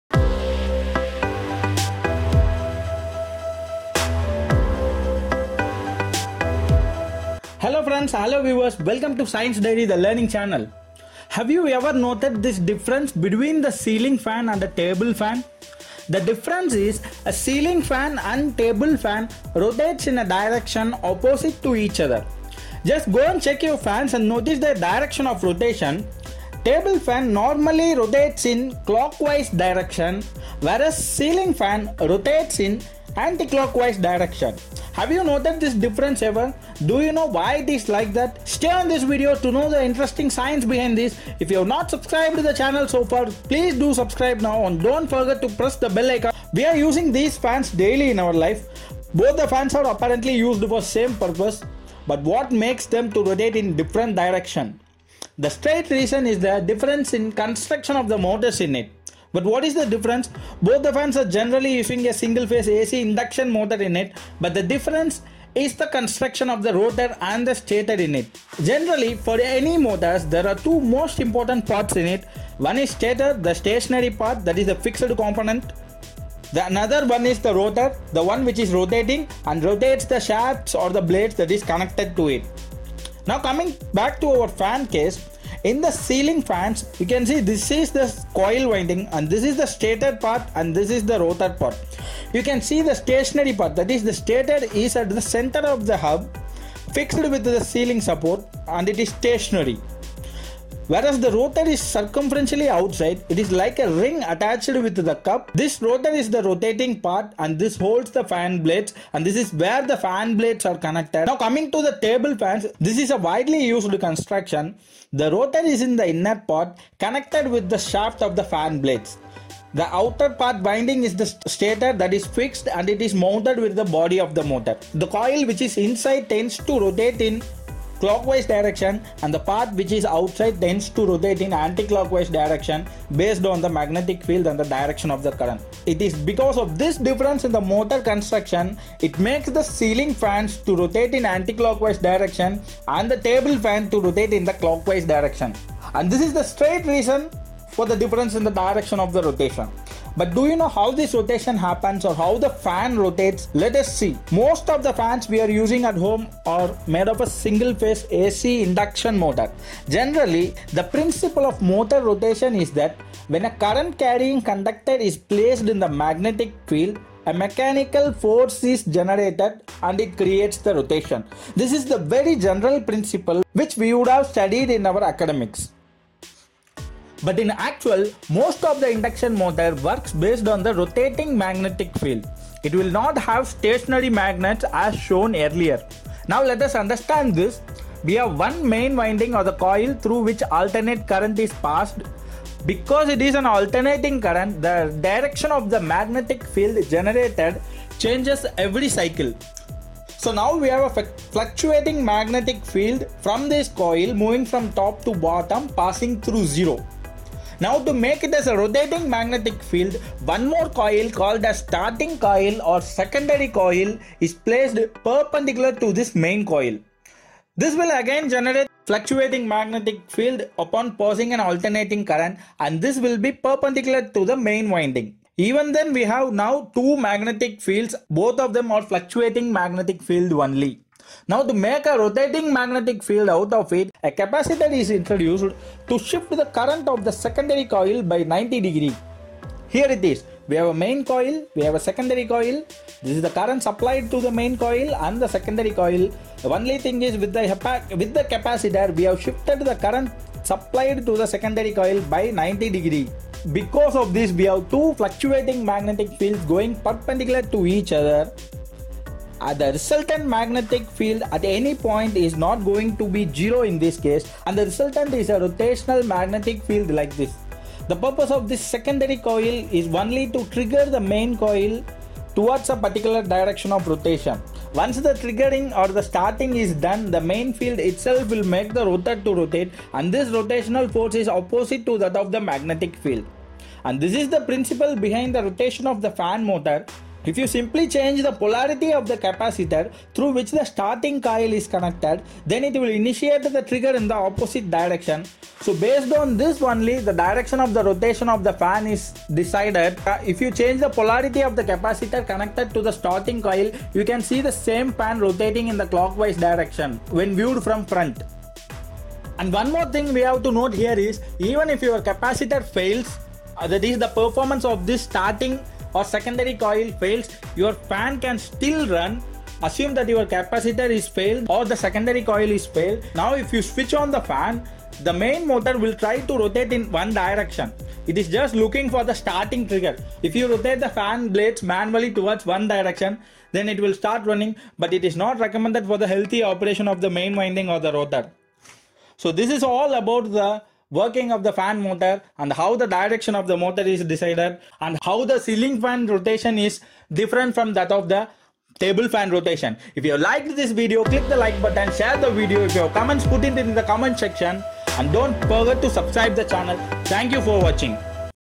Ceiling fan vs Table fan rotation I What's the difference? I How it works? I English.
This video is about The difference is Ceiling fan and table fan direction of rotation.
Table fan normally rotates in Clockwise direction,
whereas Ceiling fan rotates in anti-clockwise direction.
Watch this video to know the science behind this.
Both are fans are apparently used for same purpose,
but what makes them to rotate in different directions?
The straight reason is the difference in the construction of the motors in it.
Both the fans are generally using a Single Phase AC Induction motor.

But the difference is the construction of rotor and stator.
In Ceiling fans, the stationary coil winding, the stator is at the centre of the hub fixed with the ceiling supports and it is stationary.
Whereas the rotor is circumferential outside. It is like a ring attached with the cup that holds the fan blades. And this rotates the blades.
It is this difference in the motor construction that makes the Ceiling fan
to rotate in anti-clockwise direction and Table fan in the clockwise direction.
Generally, the principle of motor rotation is that, when a current carrying conductor is placed in the magnetic field a mechanical force is generated and it creates the rotation.
But in actual, most of the induction motors works based on the rotating magnetic field.(RMF)
We have one main winding / coil through which alternate current is passed.
Because it is alternating current, the direction of magnetic field changes
every cycle.
Now to make it as a rotating magnetic field, one more coil
called as starting coil or secondary coil is placed
perpendicular to the main coil.
Even then we have two fluctuating magnetic fields only,
Now to make a rotating magnetic field out of it,
a capacitor is introduced to shift the current to the secondary coil
by 90 Degree.
And the resultant is a rotating magnetic field like this.
The purpose of this Secondary coil is only trigger the main coil
field to a rotational direction.
Once triggered/started, the main field itself will make the rotor to
rotate and this rotational force is opposite to that of magnetic field.
If you change the polarity of the capacitor through which the
starting coil is connected, then it will initiate the trigger in opposite
direction.